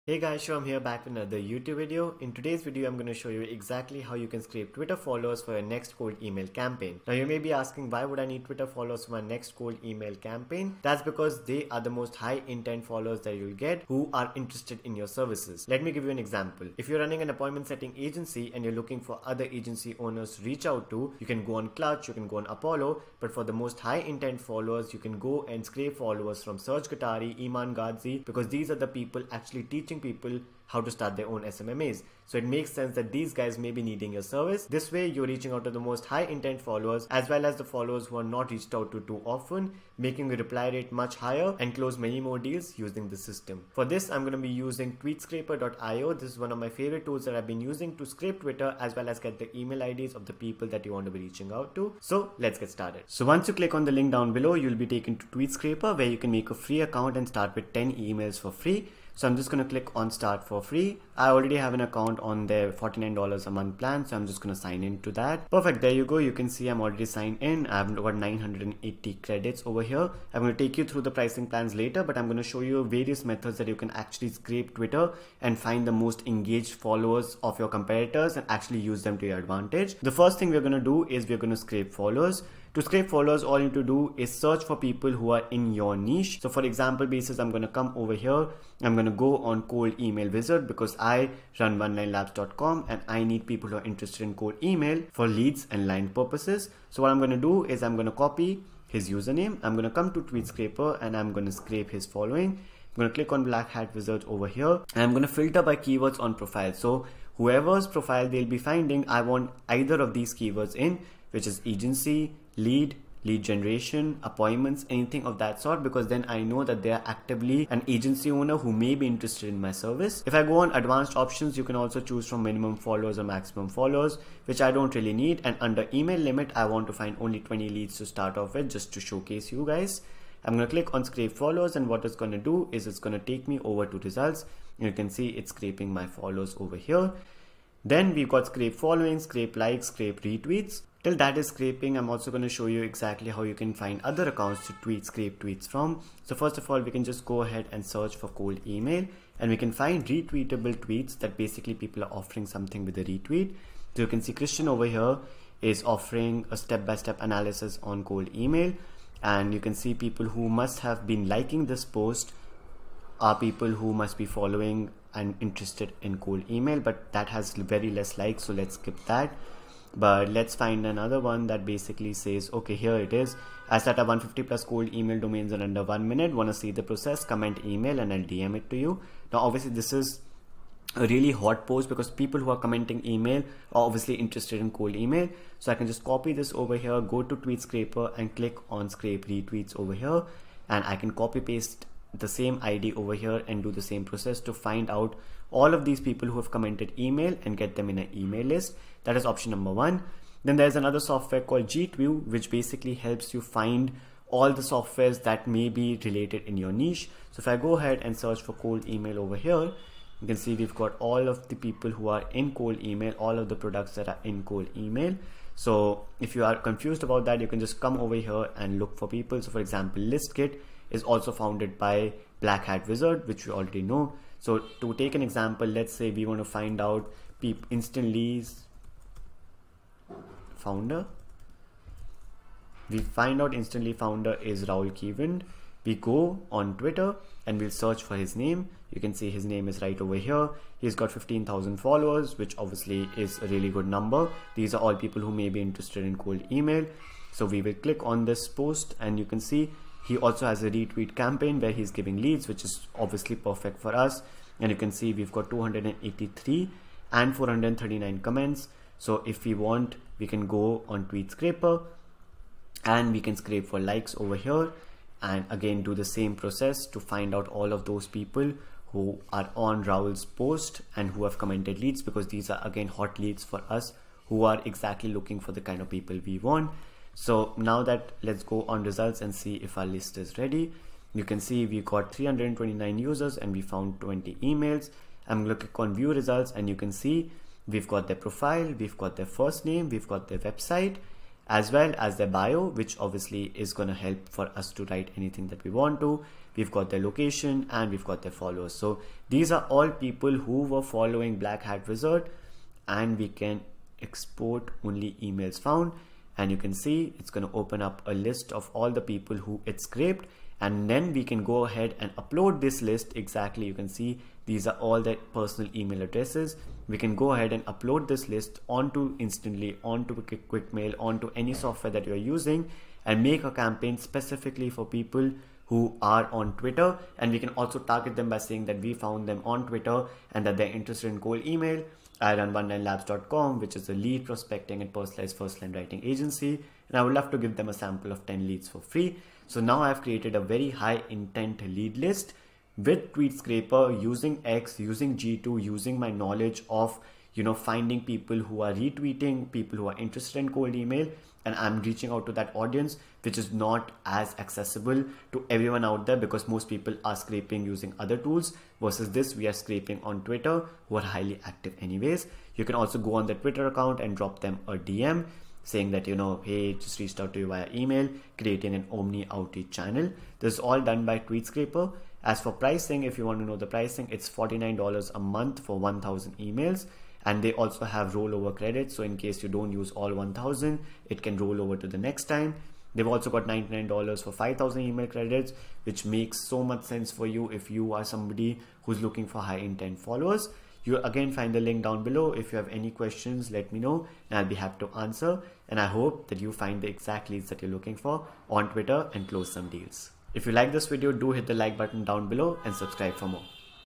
How to Scrape 1000's of Targeted Emails From Twitter in 2024 | TweetScraper
In this video, we'll be going through how you can use twitter to 2x your meeting and closing rate.